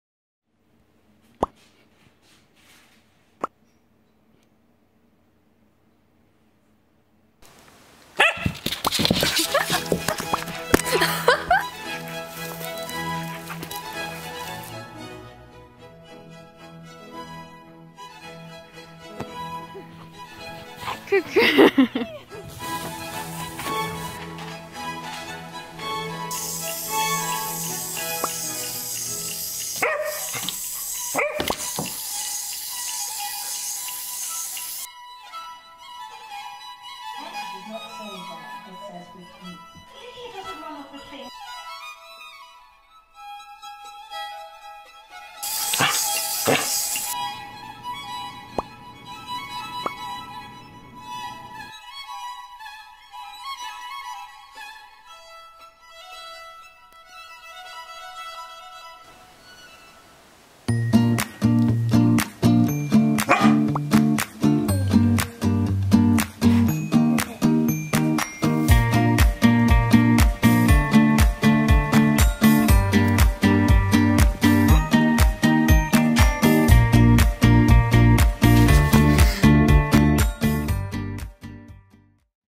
Are poodles stuck up?
3 reasons why poodles don't fit the stereotype